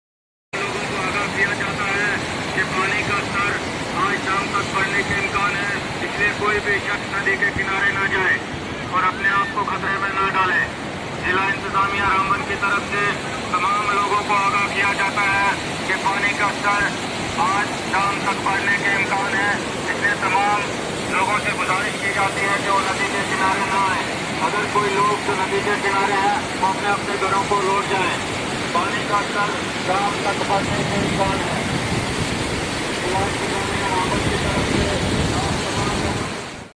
जम्मू के किश्तवाड़ में बादल फटने से 6 की मौत, 35 से अधिक लापता | ASD News
Jammu Kashmir में लगातार भारी बारिश हो रही है. इस बीच जम्मू-कश्मीर के किश्तवाड़ से बादल फटने की खबर आई है. किश्तवाड़ जिले के एक सुदूर गांव में बुधवार तड़के बादल फटने की घटना में 6 की मौत, 30 से अधिक लोग लापता हो गए